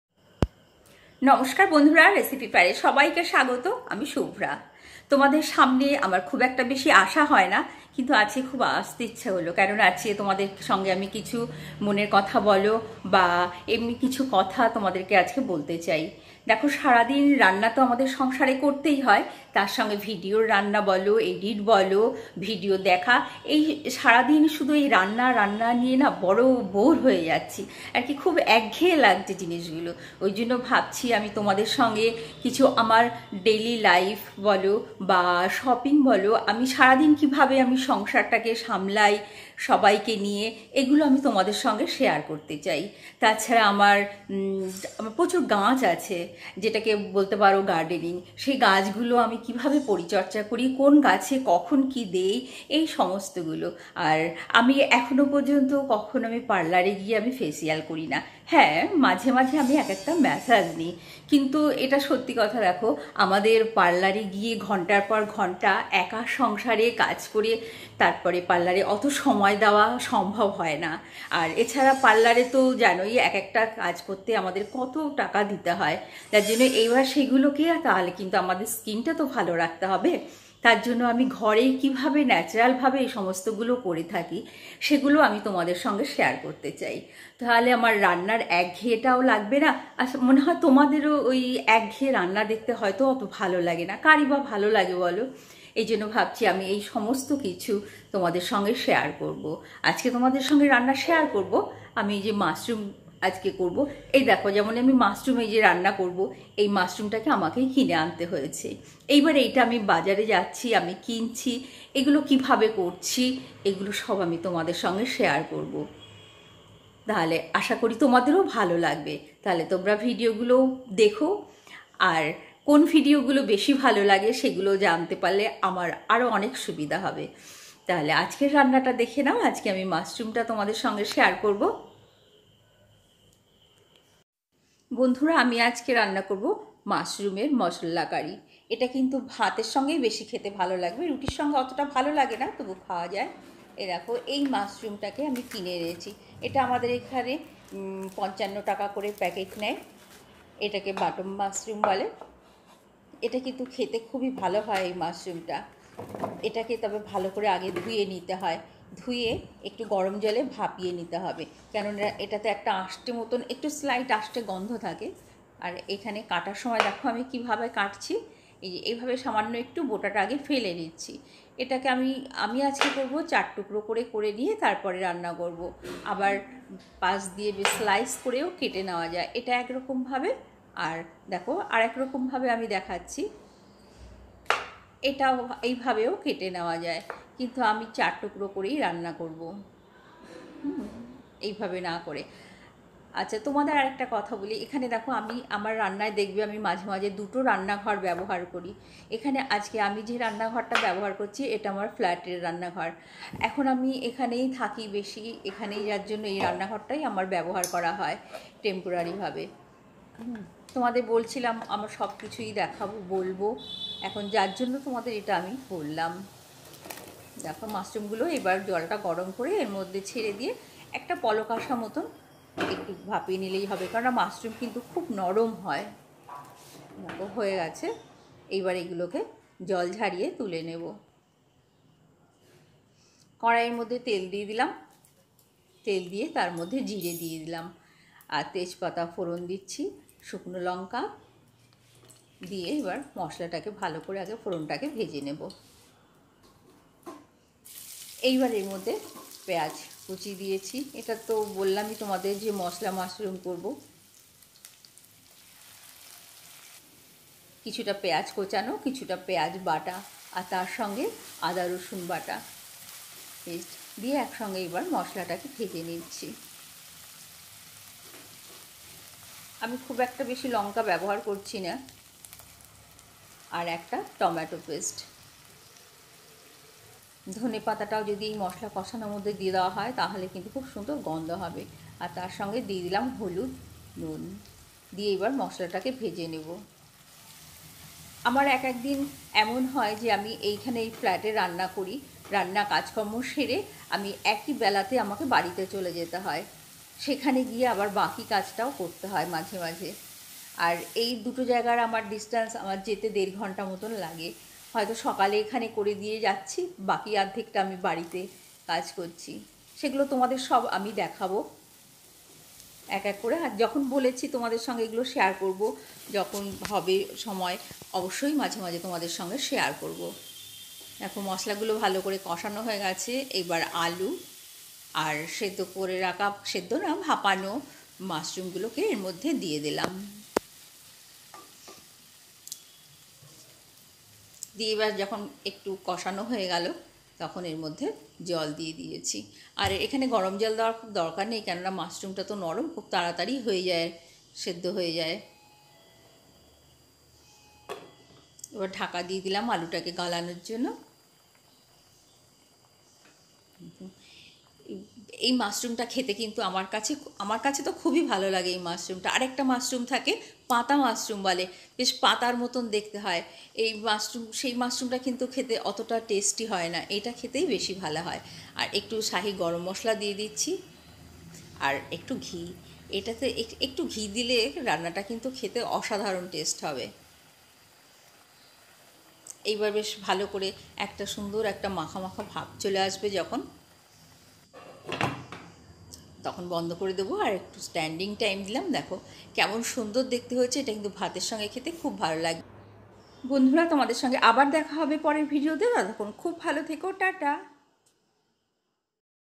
কিছু মনের কথা সাথে মসলা মাশরুম কারি /masala mushroom curry/@recipefair
বন্ধুরা কেমন আছো তোমাদের সঙ্গে আজ কিছু কথা বললাম। সঙ্গে মাশরুম রেসিপি শেয়ার করলাম। এবার থেকে আমি একটু অন্যভাবেই তোমাদের সঙ্গে ভিডিও শেয়ার করব। ভালো লাগলে অবশ্যই সঙ্গে থেকো আর কমেন্ট করে জানিও।